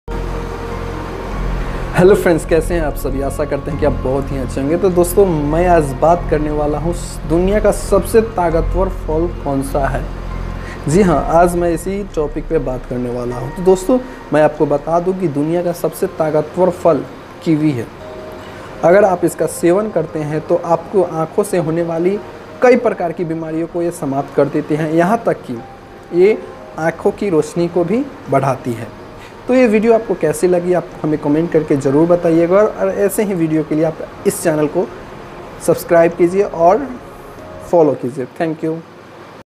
🖋🖋दुनिया का सबसे ताकतवर फल कीवी है इसका सेवन करने से आँखों से सम्बंधित बीमारियाँ ख़त्म होती है
दुनिया का सबसे ताकतवर फल कीवी है इसका सेवन करने से आँखों से सम्बंधित बीमारियाँ ख़त्म होती है और आँखों की रोशनी बढ़ती है।